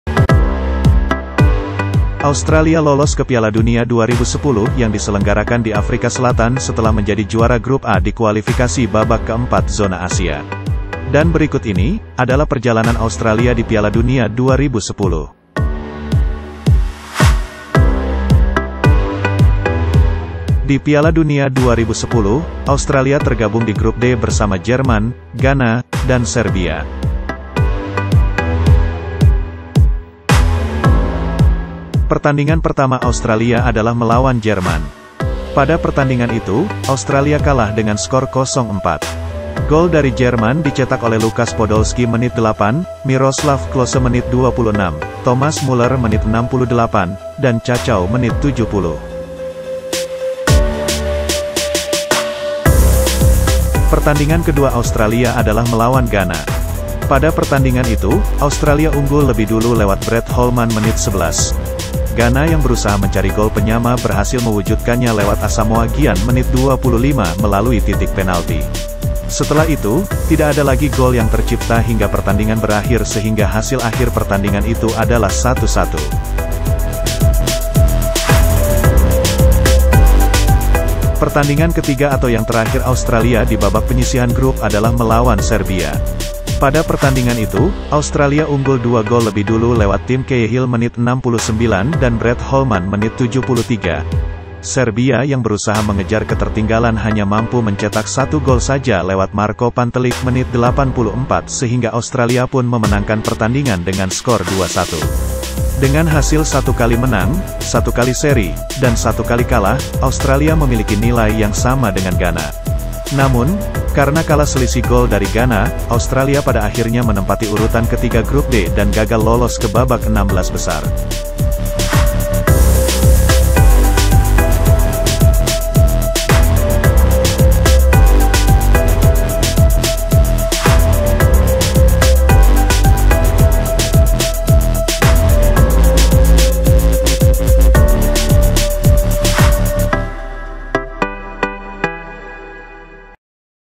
PERJALANAN AUSTRALIA DI PIALA DUNIA 2010
Halo semuanya. Pada video kali ini, kami akan membahas perjalanan australia di piala dunia 2010

Seelah berhasil lolos ke babak 16 besar pada piala dunia 2006, australia kemudian hampir saja mengulanginya di piala dunia 2010. Meskipun pada
pertandingan pertama di piala dunia 2010 mengalami kekalahan, australia kemudian mendapatkan hasil bagus di dua pertandingan berikutnya dan
berhasil mendapatkan poin yang sama dengan ghana. Namun, karena kalah selisih gol dari ghana, australia akhirnya gagal lolos ke babak 16 besar.